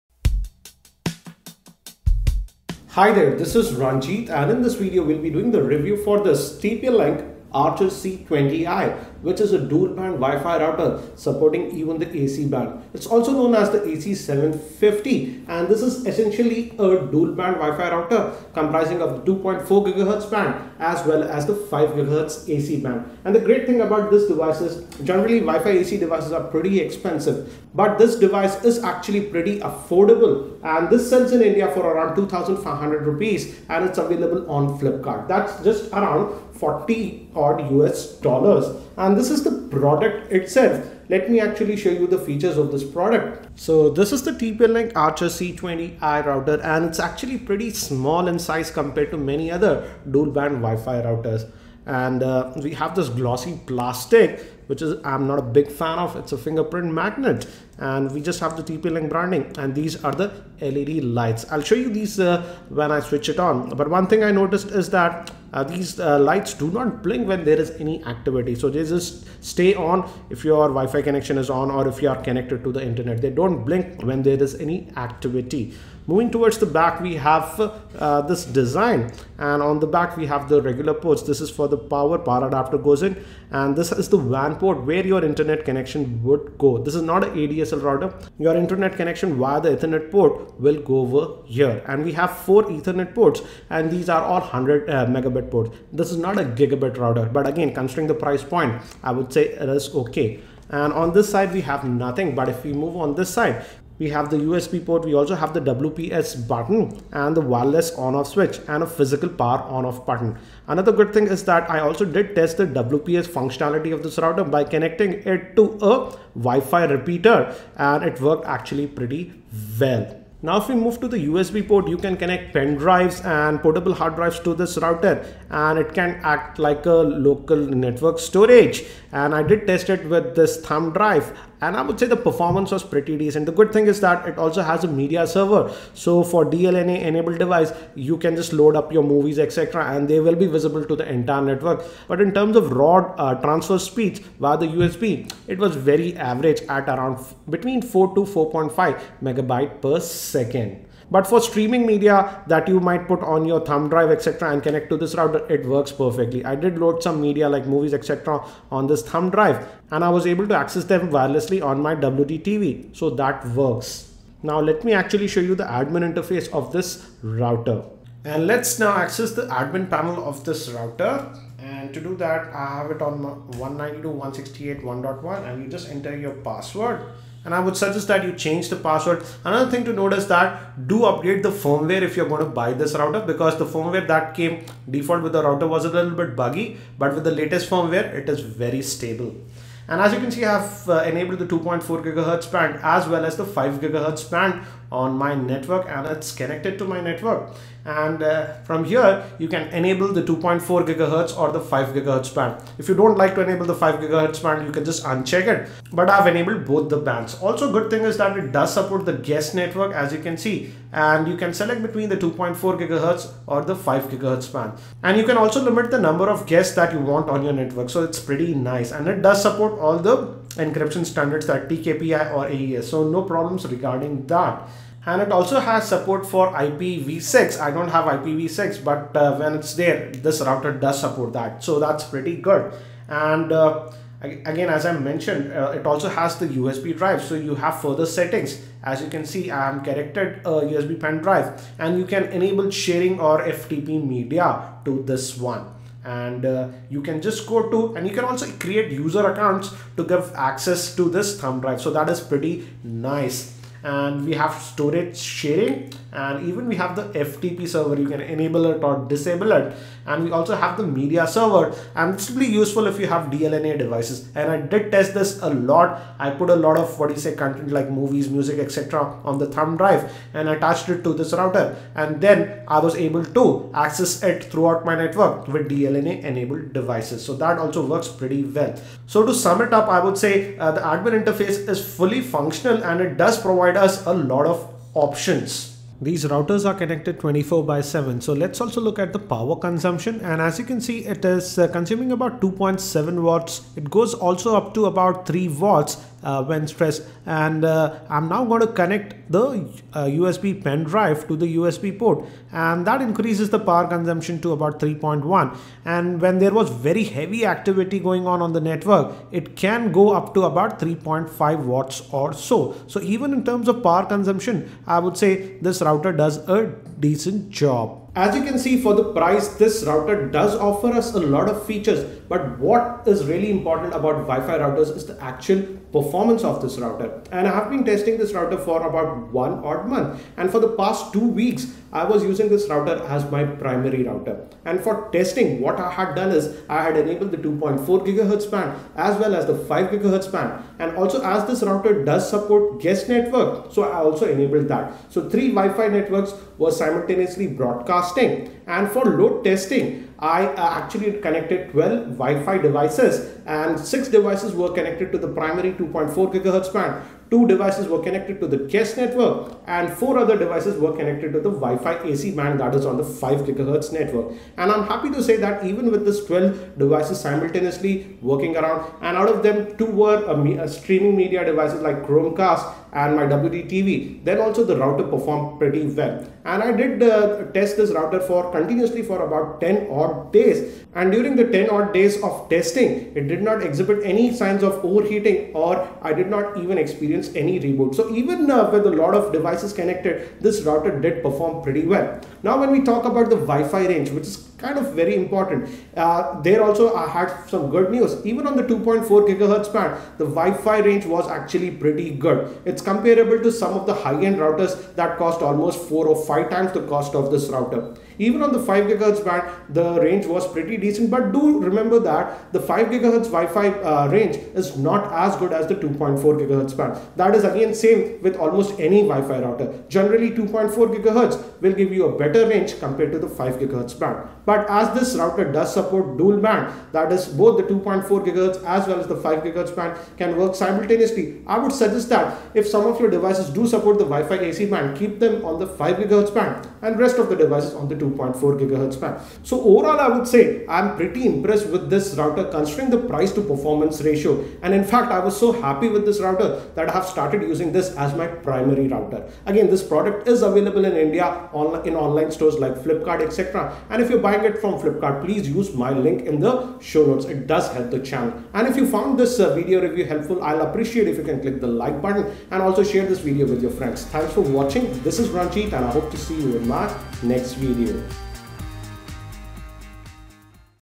TP-Link Archer C20i Budget Dual Band WiFi AC Router Review
TP-LINK Archer C20i Budget WiFi AC Router Review, Archer C20i AC750 is a simultaneous dual-band WiFi router supporting the WiFI AC band it also support the Guest Network functionality and also has USB 2.0 port and I give you an in-depth review after testing this WiFi router for about 30 days.

You can purchase the TP-Link Archer C20i Online in India via flipkart or via Amazon India